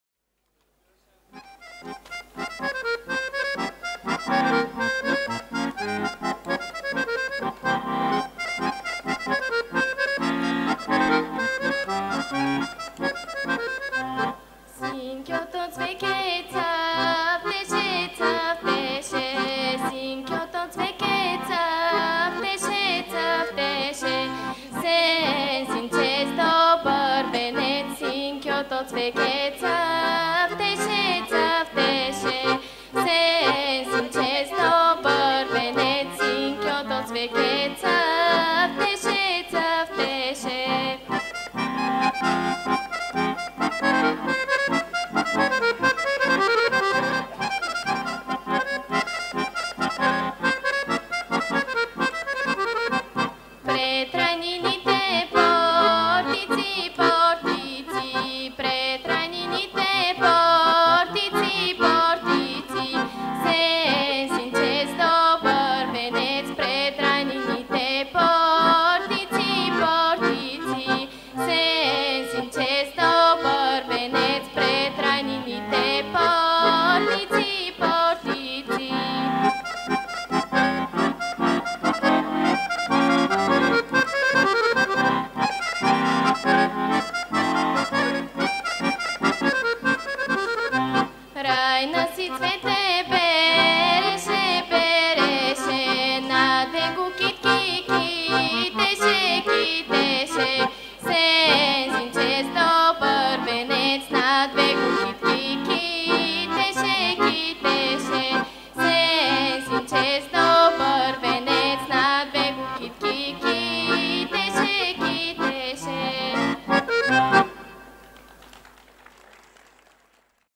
Бетина Първанова
Продуцентска къща „ПИРИНА“, Община Котел и НУФИ „Филип Кутев“ представят  първото издание на фестивала "ДА СЕ СРЕЩНЕМ С ПЕСЕН" 2014г, проведен на 15 и 16 Юни в град Котел.